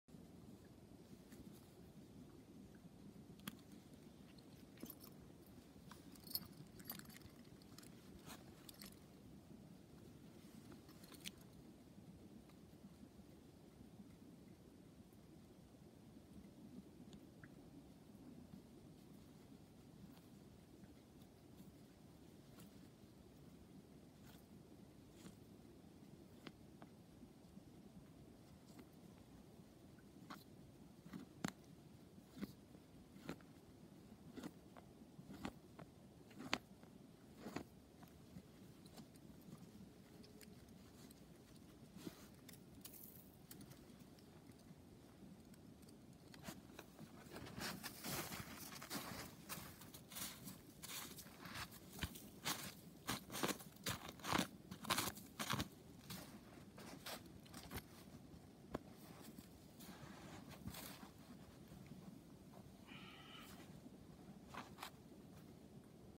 Beaver Pelt Snap Sack
That multi purpose snap sack is used as a seating pad or to carry small game out of the woods. Hand made from beaver pelt and elk hide.